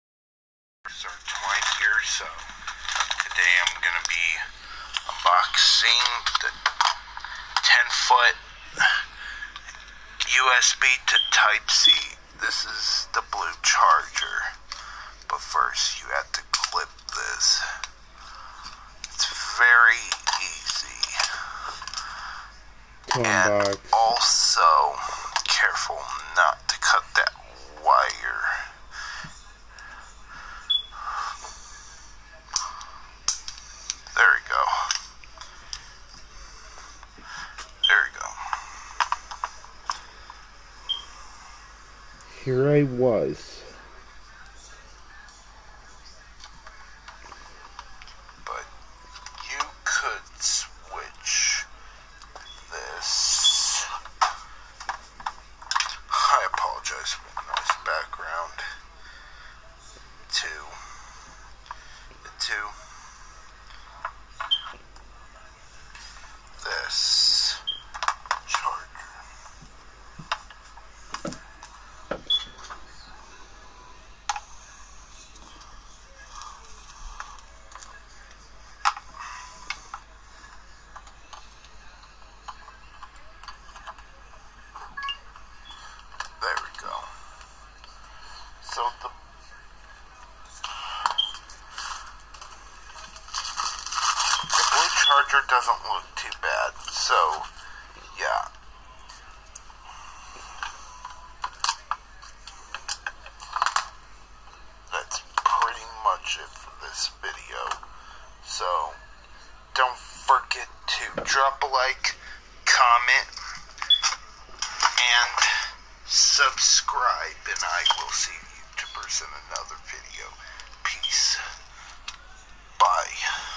me unboxing the blue charger USB to type c
I'm happy with this USB to type c I could use it as a phone charger